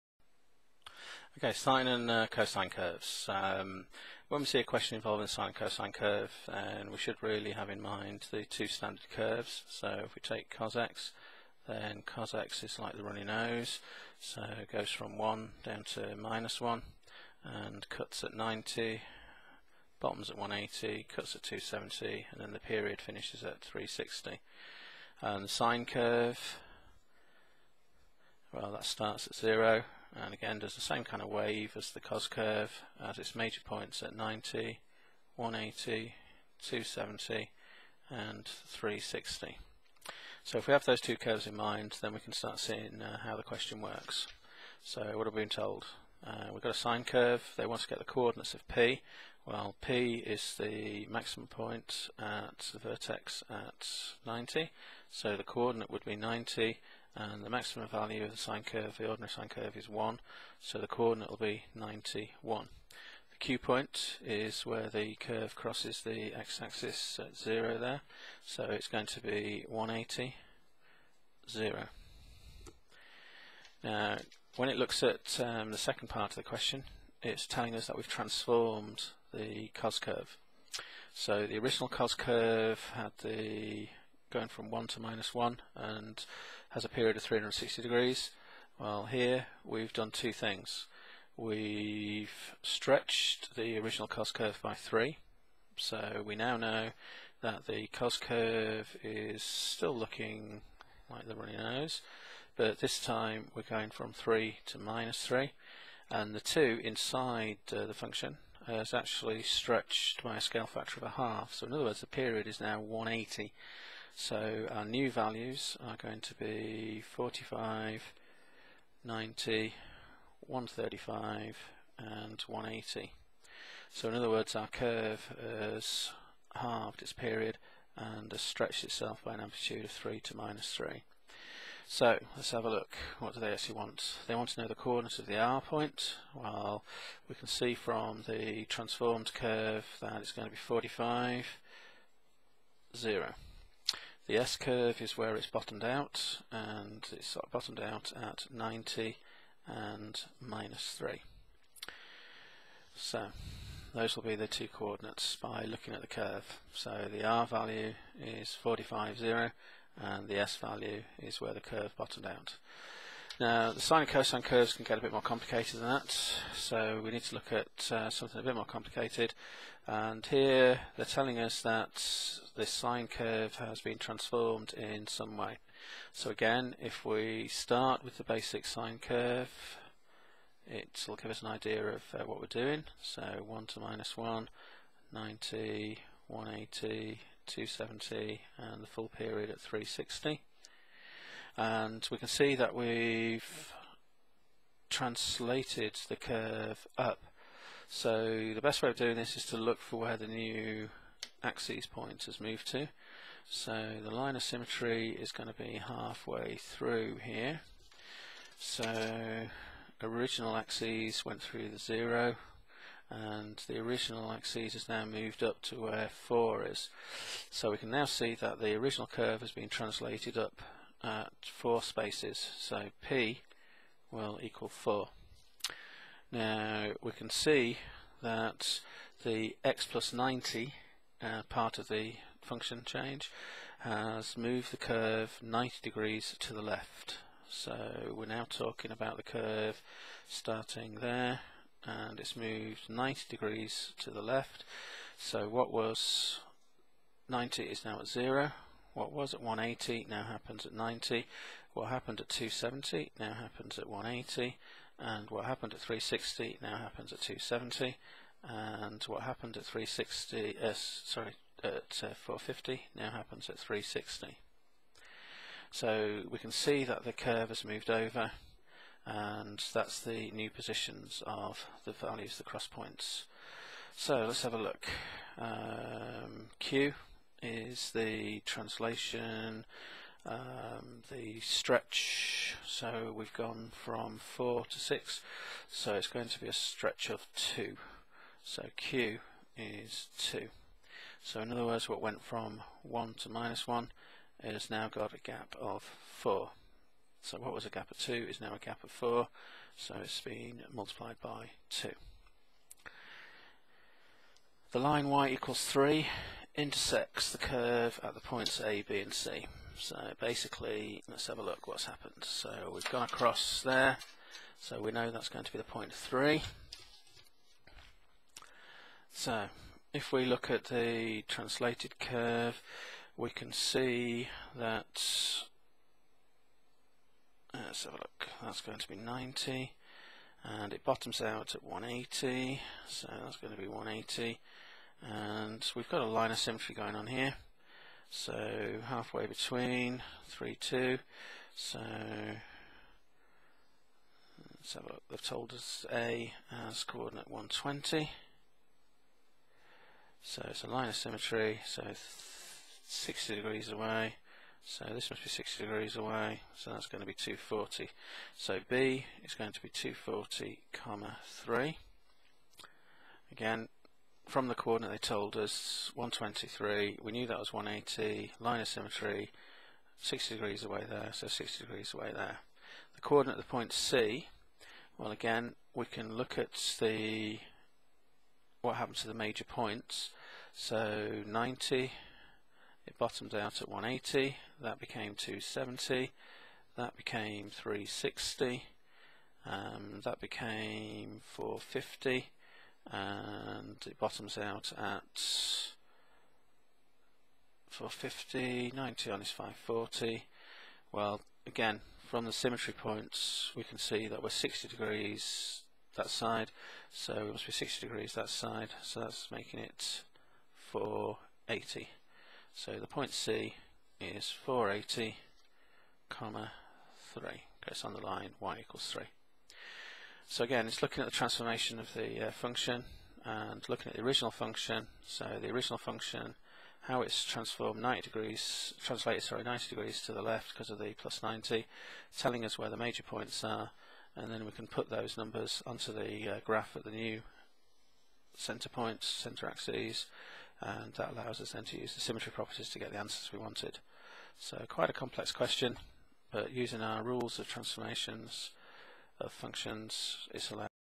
Unit 3 Higher - Cosine - Sine Curves - Transformations.wmv
Solving problems involving the sine and cosine curves and their transformations